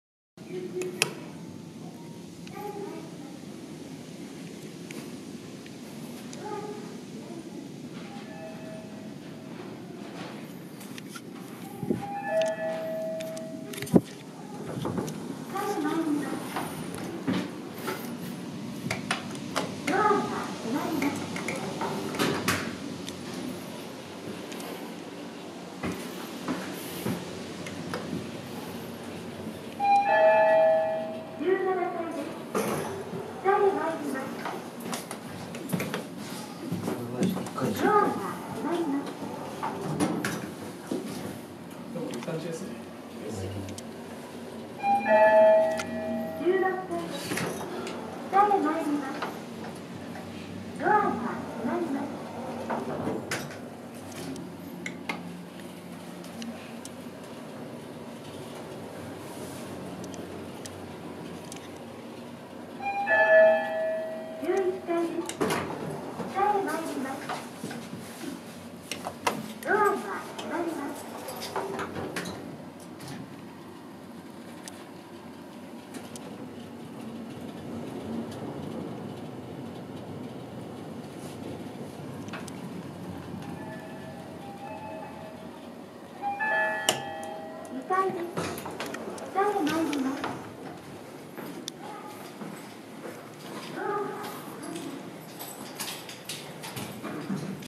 ORC200のエレベーター下り
2017年12月20日に撮影したエレベーターで、OTISのスペッククレセスです。
インジケーターが珍しく、ホールランタンはありますが、予報音はありません。
車椅子対応機がない側の右側エレベーターです。
珍しいアナウンスです。